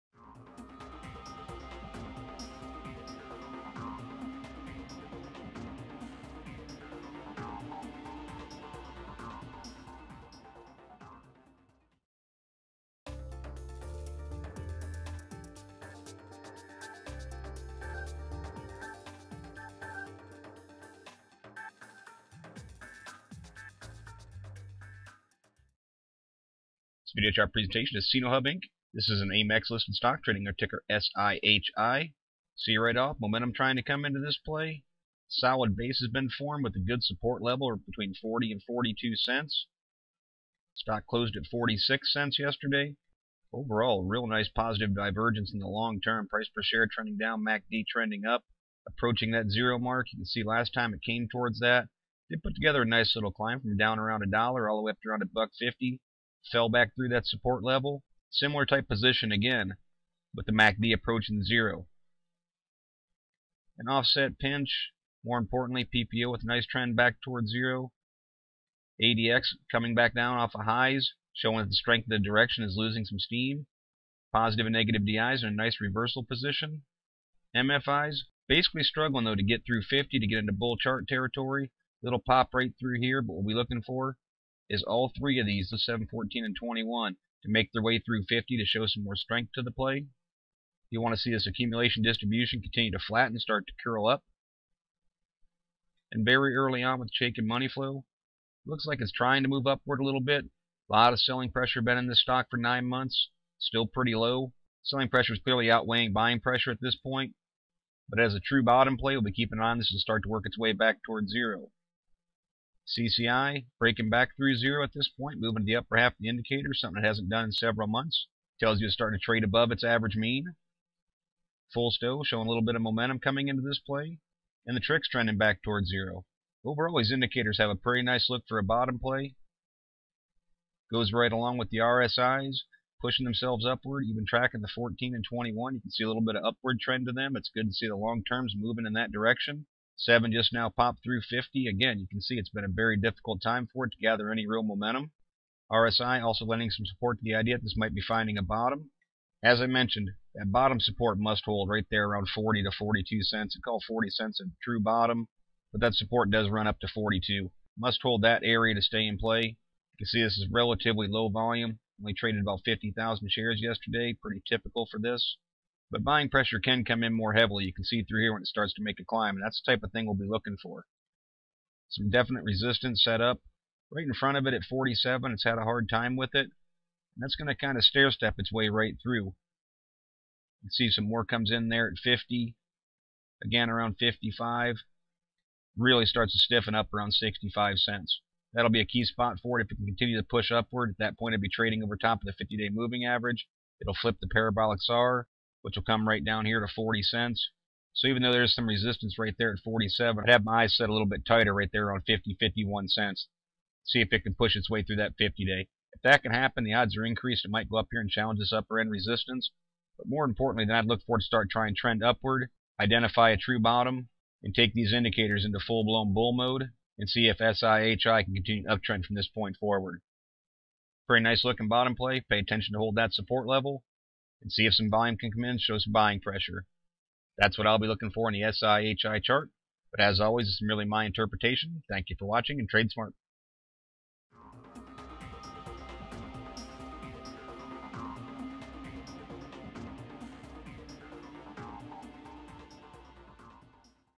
QualityStocks Video Chart SIHI 11/9/11
SIHI is holding a firm base in the area of 40 cents.  The indicators are at key points and appear to be turning bullish with the first resistance at 48 cents.  Bottom support must hold.